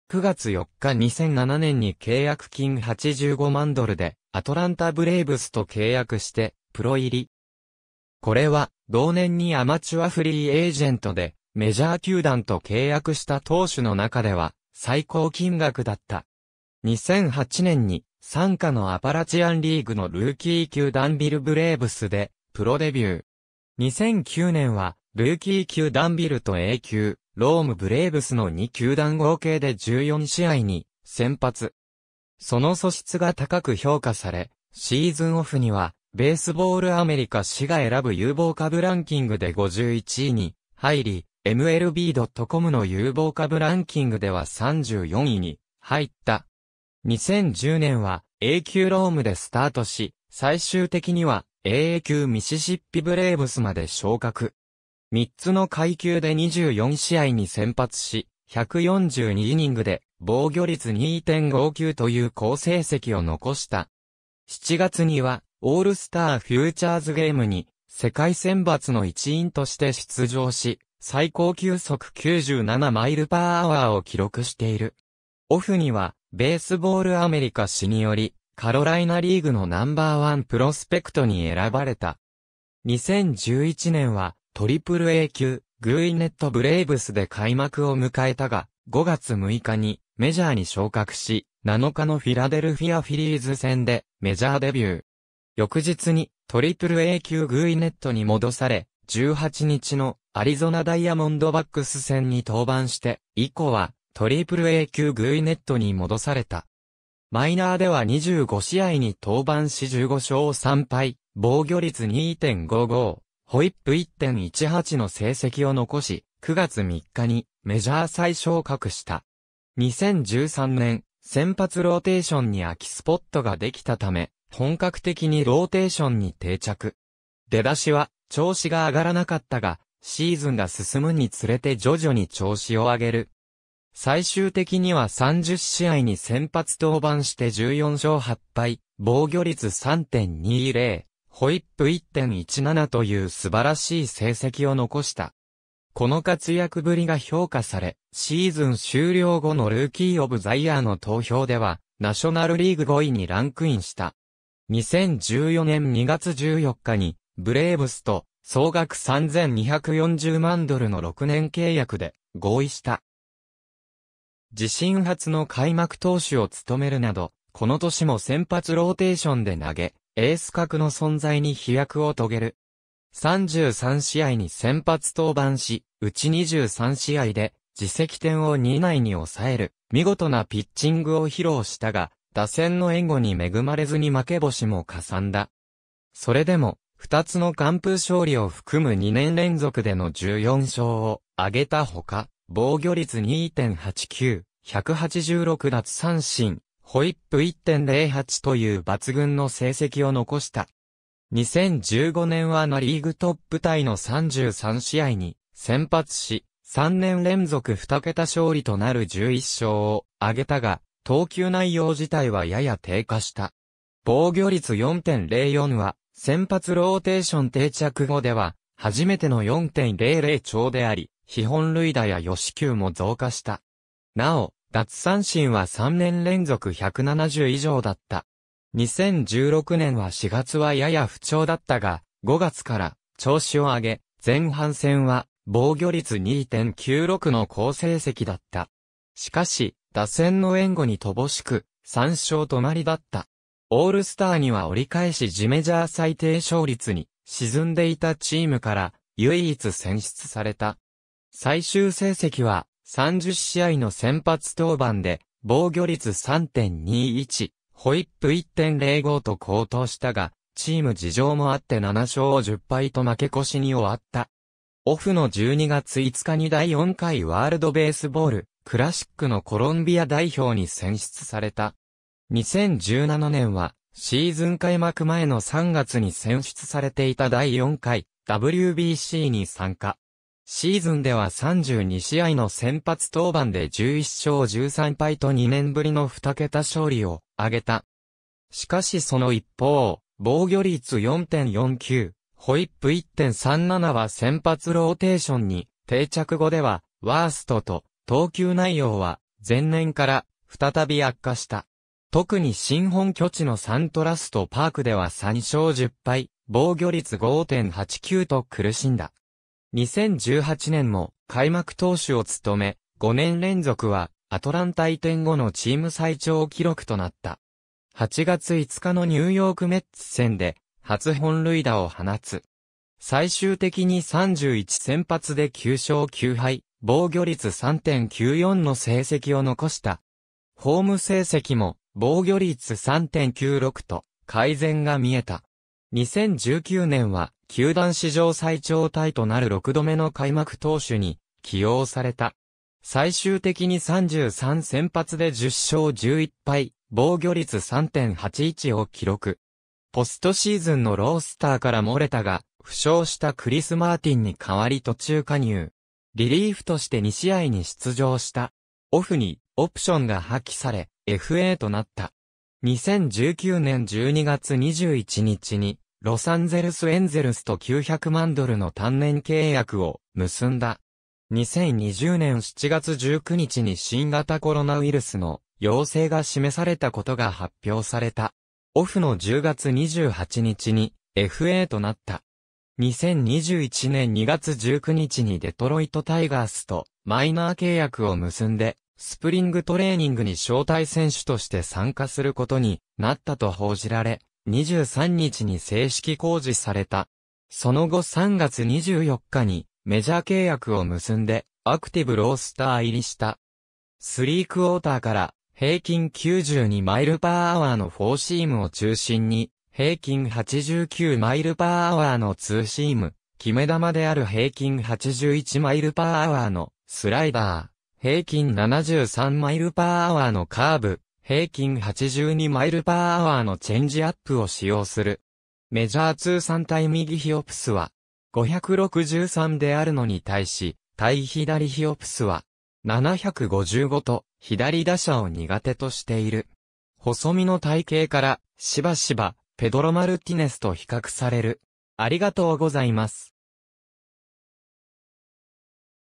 フリオ・テヘラン
ワールド・ベースボール・クラシック・コロンビア代表選手
フリオ・アルベルト・テヘラン・ピント（Julio Alberto Teheran Pinto, 1991年1月27日 - ）は、コロンビアのボリーバル県カルタヘナ出身のプロ野球選手（投手）。
右投右打。
MLBのデトロイト・タイガース所属。
2015年9月4日 2007年に契約金85万ドルでアトランタ・ブレーブスと契約してプロ入り。
これは同年にアマチュア・フリーエージェントでメジャー球団と契約した投手の中では最高金額だった。
2008年に傘下のアパラチアンリーグのルーキー級ダンビル・ブレーブスでプロデビュー。
2009年はルーキー級ダンビルとA級ローム・ブレーブスの2球団合計で14試合に先発。
その素質が高く評価され、シーズンオフには「ベースボール・アメリカ」誌が選ぶ有望株ランキングで51位に入り、MLB.comの有望株ランキングでは34位に入った。
2010年はA級ロームでスタートし、最終的にはAA級ミシシッピ・ブレーブスまで昇格。
3つの階級で24試合に先発し、142イニングで防御率2.59という好成績を残した。
7月にはオールスター・フューチャーズゲームに世界選抜の一員として出場し、最高球速97mph（約156.1km/h）を記録している。
オフには、「ベースボール・アメリカ」誌により、カロライナリーグのNo.1プロスペクトに選ばれた。
2011年はAAA級グウィネット・ブレーブスで開幕を迎えたが、5月6日にメジャーに昇格し、7日のフィラデルフィア・フィリーズ戦でメジャーデビュー。
翌日にAAA級グウィネットに戻され、18日のアリゾナ・ダイヤモンドバックス戦に登板して以降はAAA級グウィネットに戻された。
マイナーでは25試合に登板し15勝3敗、防御率2.55、WHIP1.18の成績を残し、9月3日にメジャー再昇格した。
2013年、先発ローテーションに空きスポットが出来た為、本格的にローテーションに定着。
出だしは調子が上がらなかったが、シーズンが進むにつれて徐々に調子を上げる。
最終的には30試合に先発登板して14勝8敗、防御率3.20、WHIP1.17という素晴らしい成績を残した。
この活躍ぶりが評価され、シーズン終了後のルーキー・オブ・ザ・イヤーの投票では、ナショナルリーグ5位にランクインした。
2014年2月14日にブレーブスと総額3240万ドルの6年契約（2020年・1200万ドルのオプション付き）で合意した。
自身初の開幕投手を務めるなど、この年も先発ローテーションで投げ、エース格の存在に飛躍を遂げる。
33試合に先発登板し、うち23試合で自責点を2以内に抑える見事なピッチングを披露したが、打線の援護に恵まれずに負け星もかさんだ。
それでも、2つの完封勝利を含む2年連続での14勝を挙げたほか、防御率2.89、186奪三振、WHIP1.08という抜群の成績を残した。
2015年はナ・リーグトップタイの33試合に先発し、3年連続二桁勝利となる11勝を挙げたが、投球内容自体はやや低下した。
防御率4.04は、先発ローテーション定着後では初めての4.00超であり、被本塁打や与四球も増加した。
なお、奪三振は3年連続170以上だった。
2016年は4月はやや不調だったが、5月から調子を上げ、前半戦は防御率2.96の好成績だった。
しかし打線の援護に乏しく、3勝止まりだった。
オールスターには折り返し時メジャー最低勝率に沈んでいたチームから唯一選出された。
最終成績は、30試合の先発登板で防御率3.21、WHIP1.05 (規定投球回に達した直近4年でベスト) と好投したが、チーム事情もあって7勝10敗と負け越しに終わった。
オフの12月5日に第4回ワールド・ベースボール・クラシック(WBC)のコロンビア代表に選出された。
2017年はシーズン開幕前の3月に選出されていた第4回WBCに参加。
シーズンでは32試合の先発登板で11勝13敗と2年ぶりの二桁勝利を挙げた。
しかしその一方、防御率4.49、WHIP1.37は先発ローテーションに定着後ではワーストと、投球内容は前年から再び...